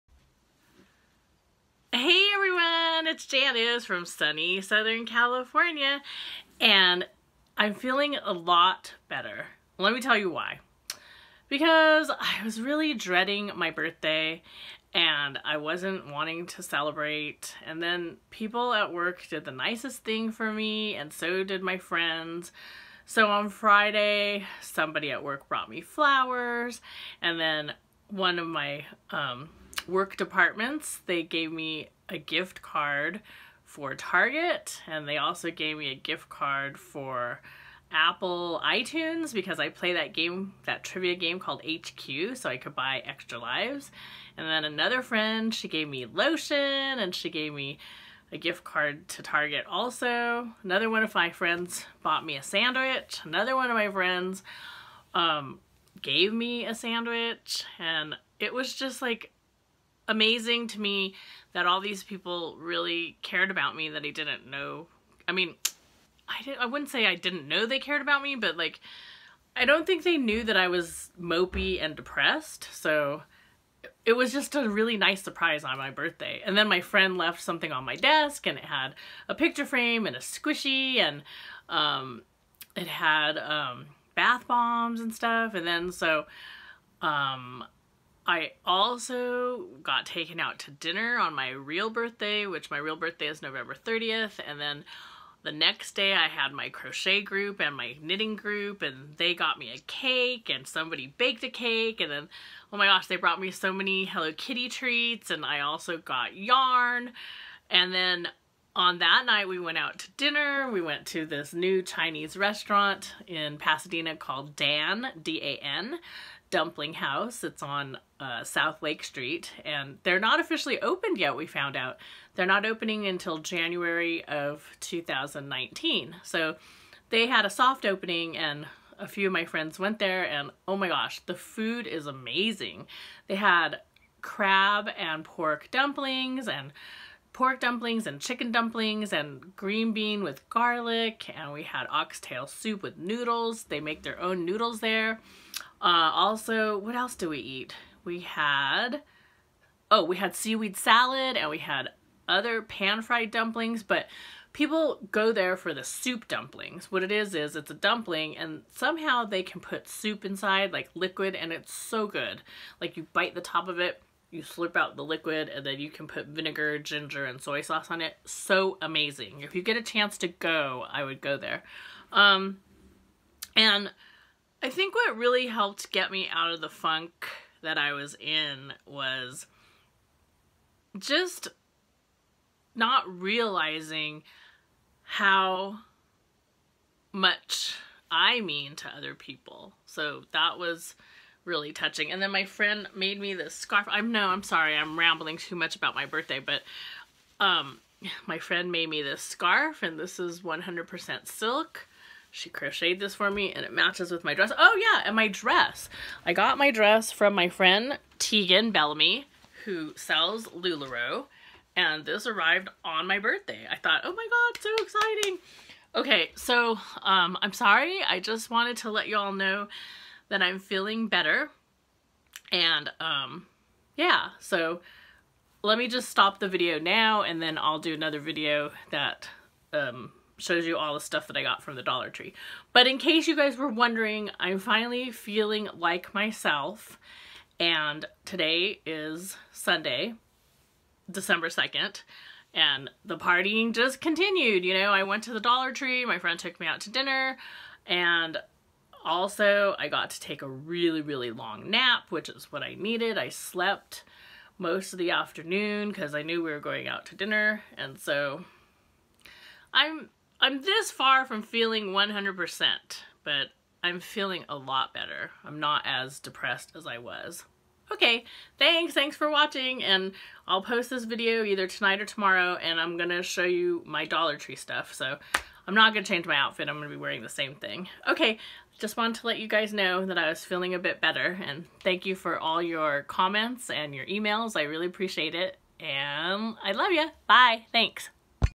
Janice talks about Birthday
Hello everyone, my actual Birthday is November 30th.  But I start celebrating early, mainly because it usually falls on Thanksgiving weekend.

I talk about what I did with my friends, and how I'm starting to feel a little bit better.  I want to thank my YouTube family for continuing to watch, and comment on my videos.  Thanks so much for your support.

Thanks and please remember to be nice to one another, the world needs more nice, kind, sincerely people, xoxo Janice